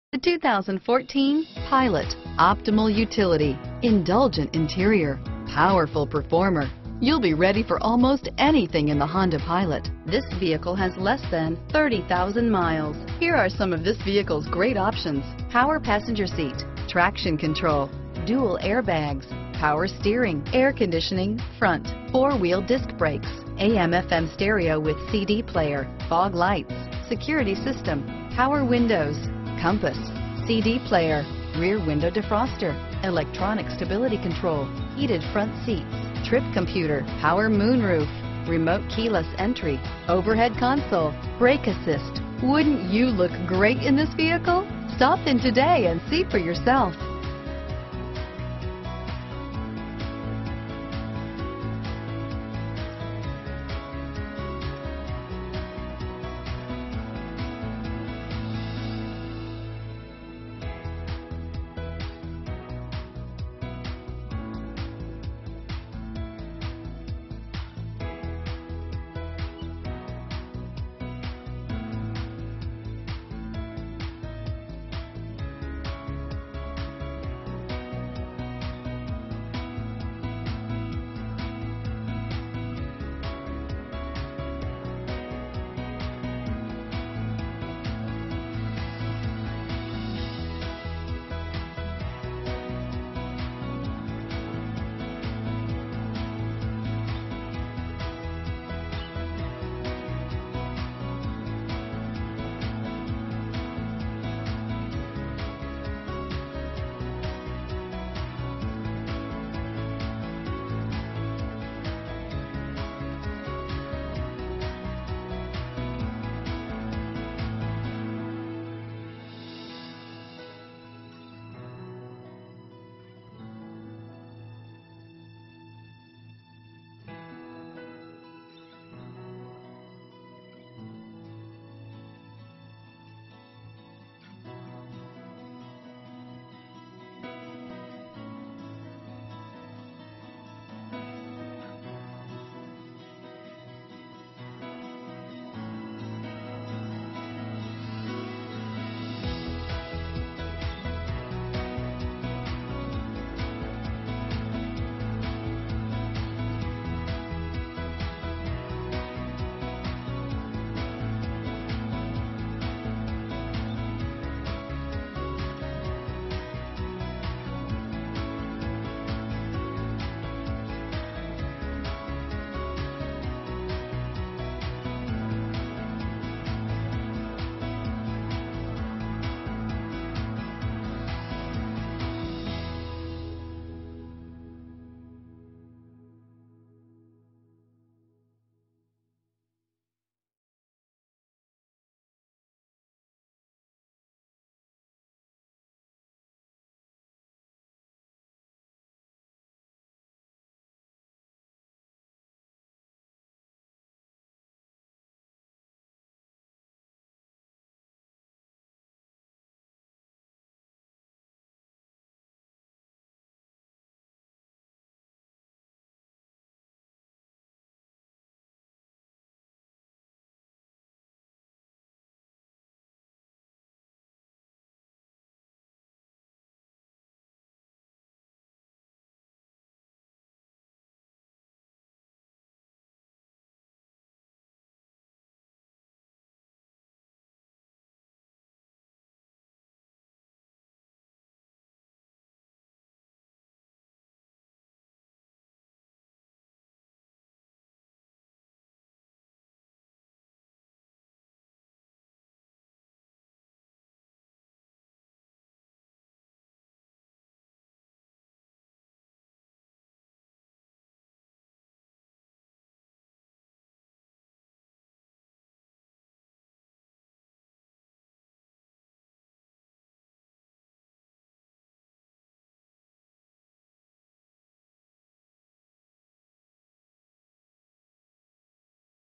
2014 Honda Pilot Reno, Sparks, Lake Tahoe, Mammoth, Northern Nevada PH1740
2014 Honda Pilot

Pilot EX-L, Honda Certified, 4D Sport Utility, 3.5L V6 24V SOHC i-VTEC, 5-Speed Automatic, and 4WD. Switch to Michael Hohl Honda Subaru! How would you like cruising home in this outstanding-looking 2014 Honda Pilot? Sporty and fun to drive with exceptionally good fuel economy. Honda Certified Pre-Owned means you not only get the reassurance of a 12mo/12,000 mile limited warranty, but also up to a 7yr/100k mile powertrain warranty, a 182-point inspection/reconditioning.